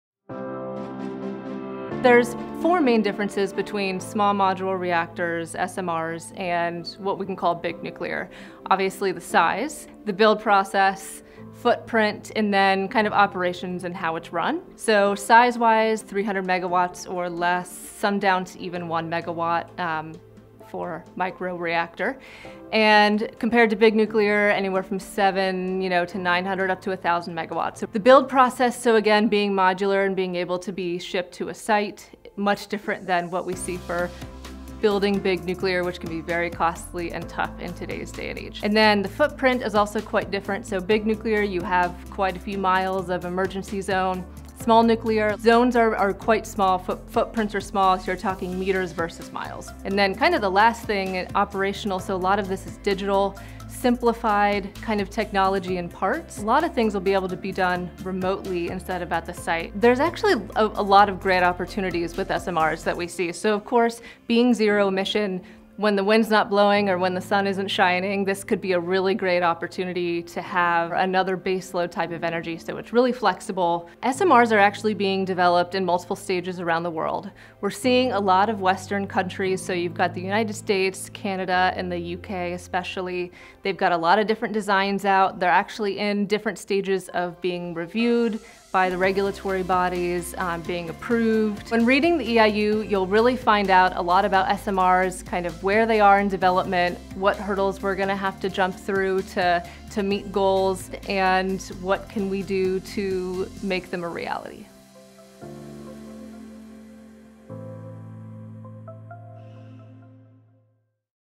ScottMadden - What are Small Modular Reactors (SMR) ? - Fall 2021 EIU
ScottMadden Management Consultants | Smart. Focused. Done Right.®

Small modular reactors compared to "big nuclear" have many advantages. Learn about the advances in SMR technology and the regulations in the latest EIU.